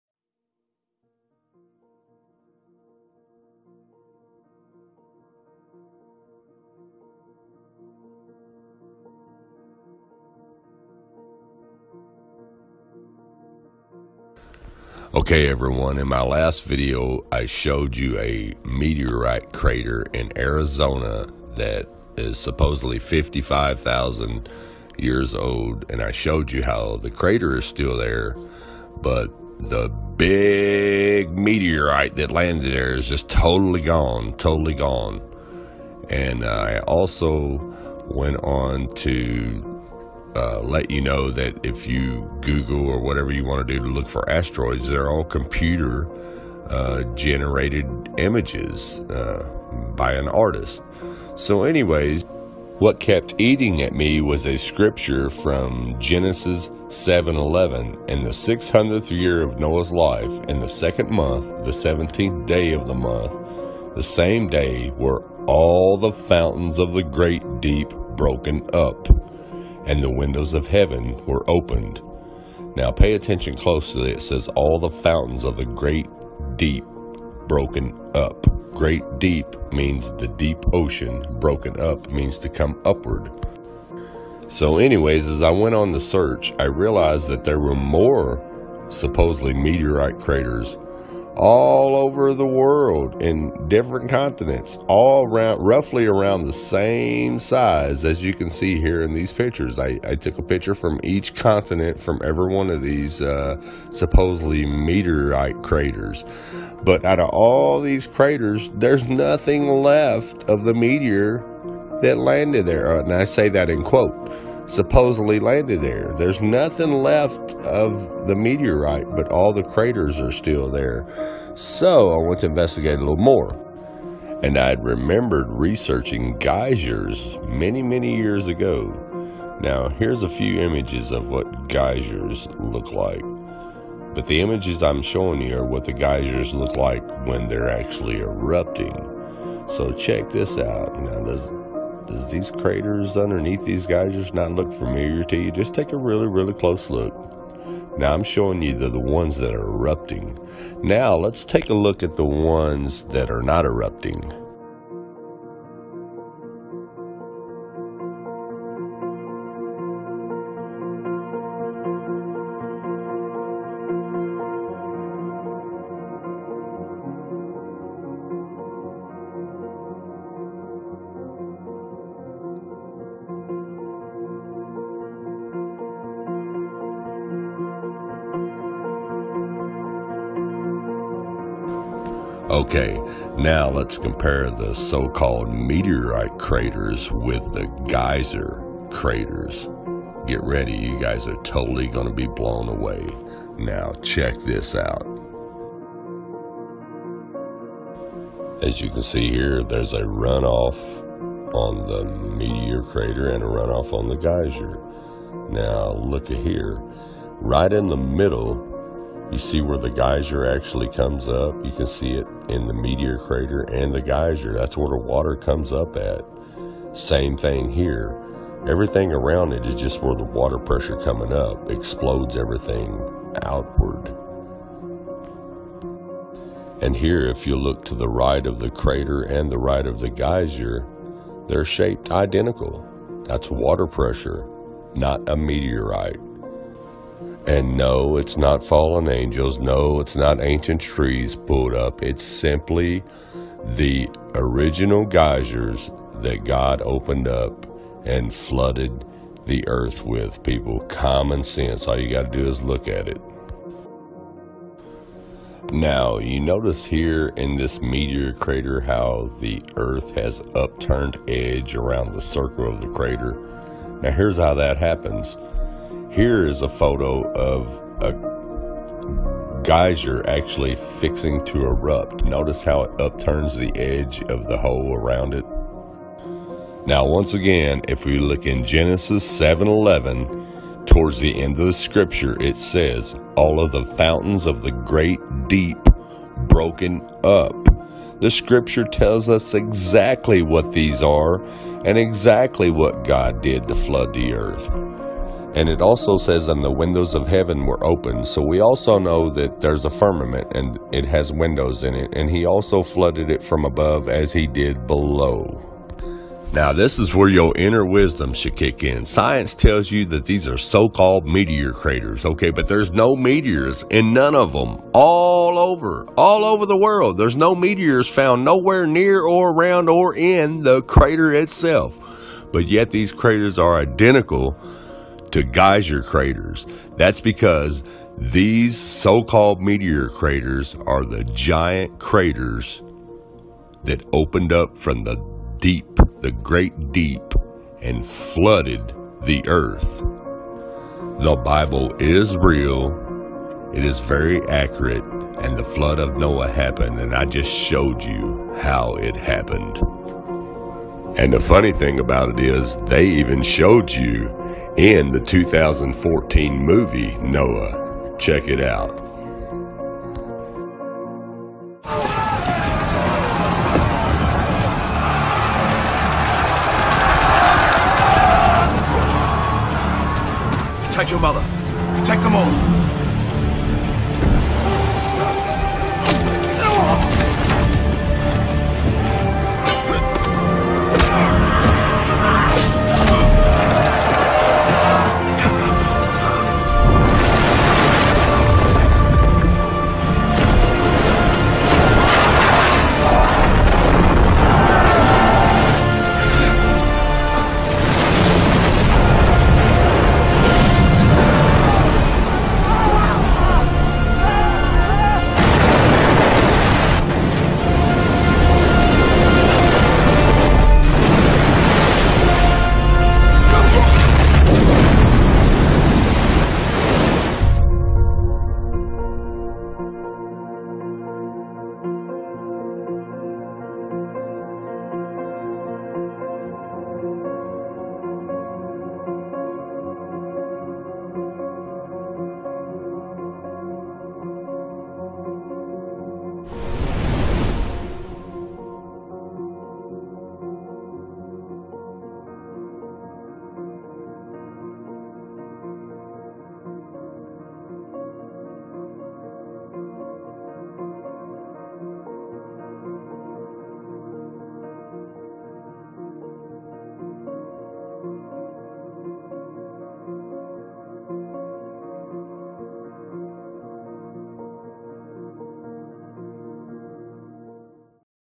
Meteor Craters, Geysers & The Great Flood by Terry Eicher
Terry Eicher’s evidence is nothing less than astonishing.  The mystery of worldwide craters has been solved with one single verse from the Bible.